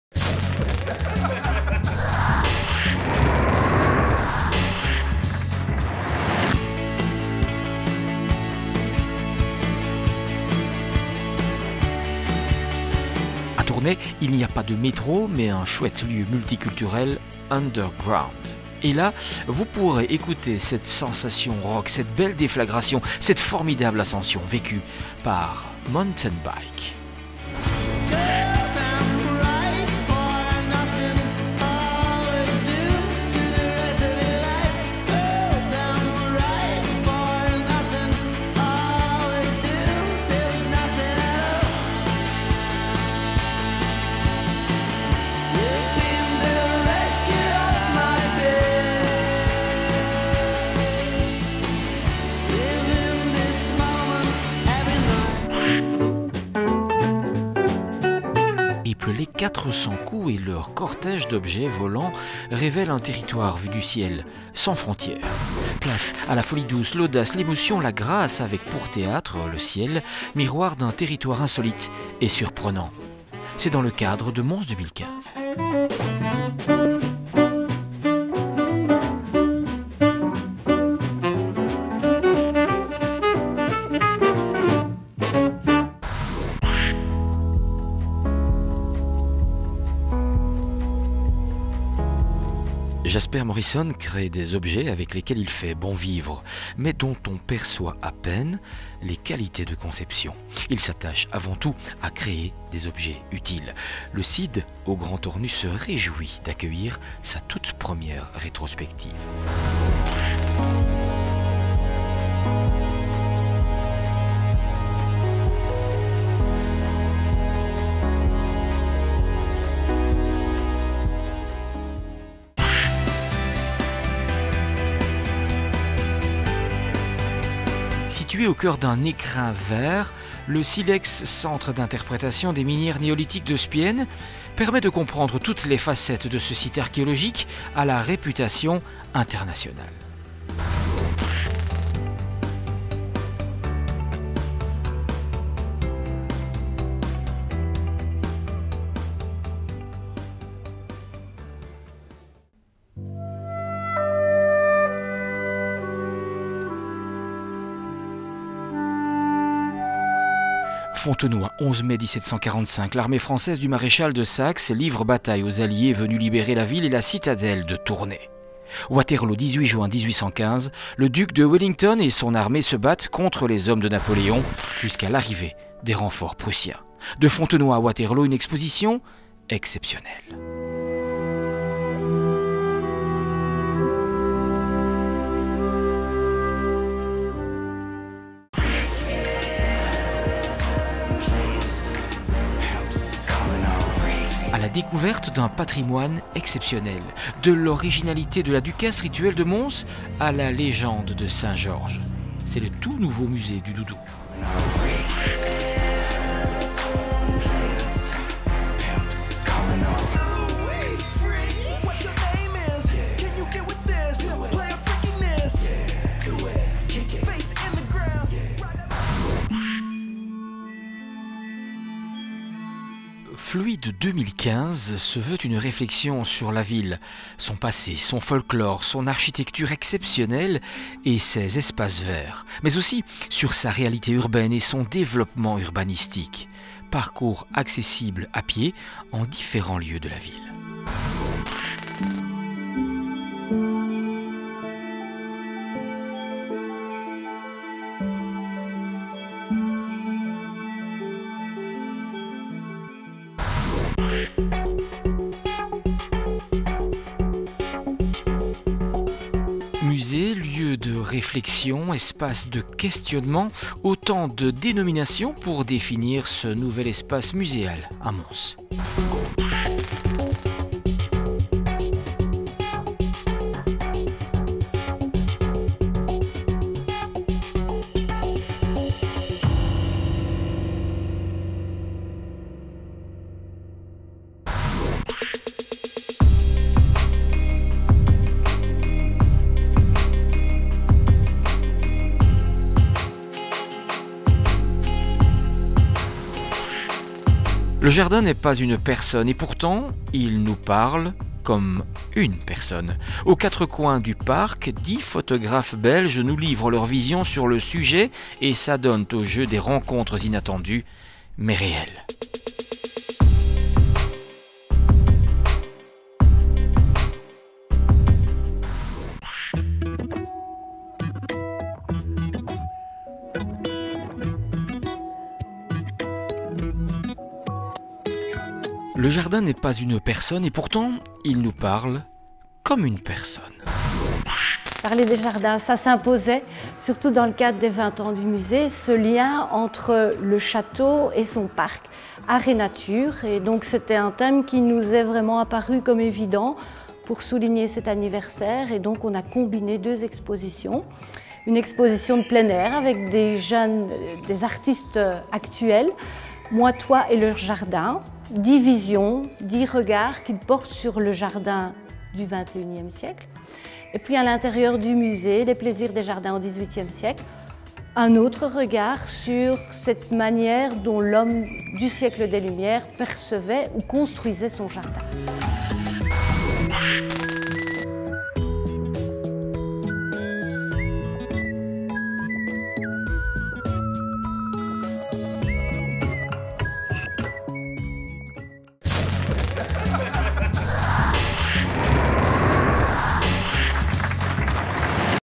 IN|OUT Hainaut n°33
IN | OUT Hainaut, magazine culturel et touristique n°33 ! Découvrez les événements In et Out de la Province de Hainaut. Cette semaine : Mountain Bike, 5 Clochers, 400 Coups, Jasper Morrison, Silex’s,… et beaucoup d’autres !